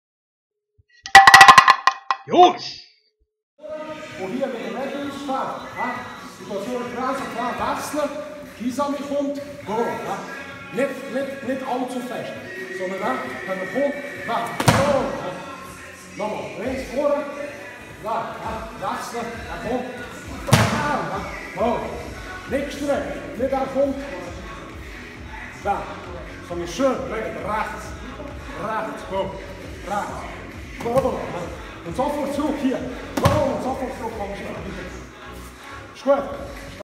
Mico Ilic Uraken-Counter
Sensei Mico Ilic shows his favorite Uraken-Counter